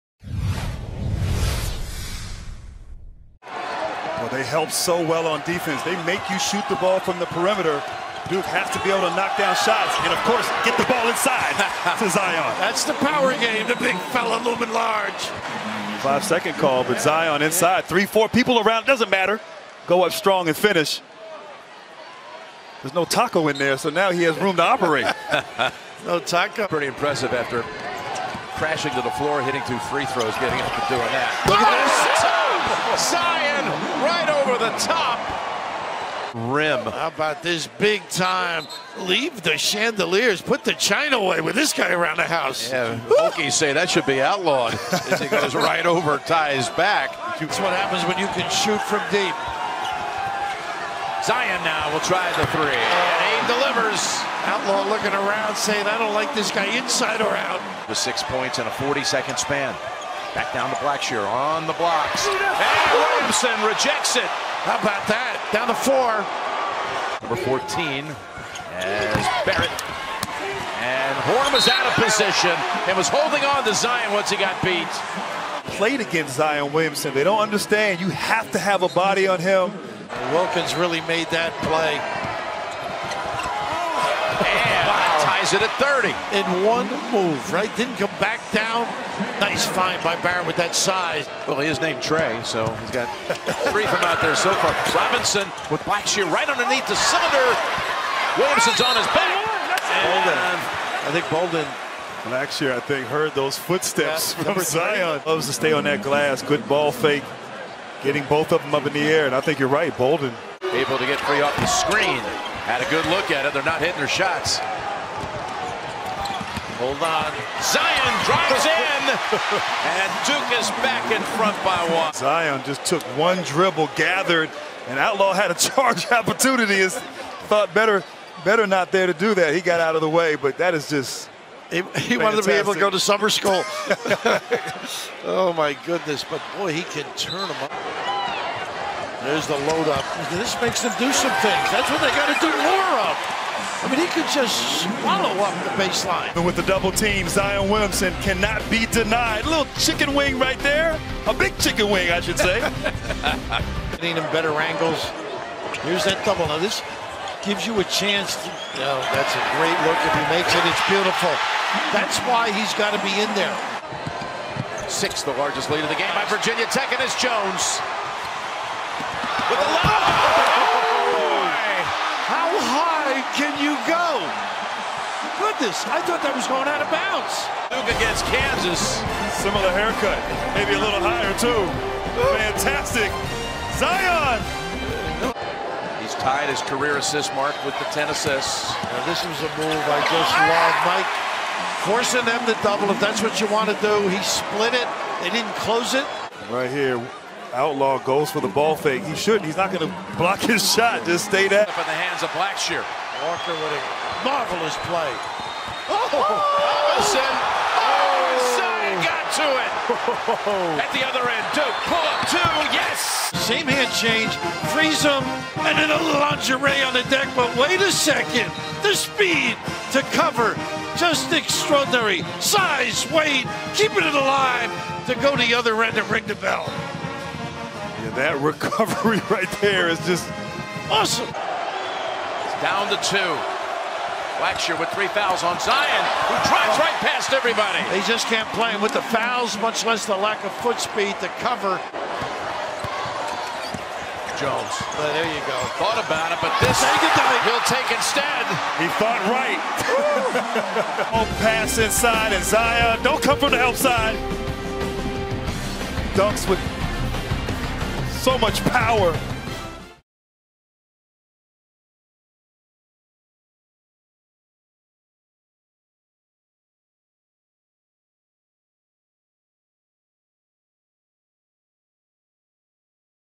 Zion Williamson's one-man highlight show in Sweet 16
Zion Williamson scores 23 points on 11-14 shooting, to go with 6 rebounds in Duke's thrilling Sweet 16 victory over Virginia Tech.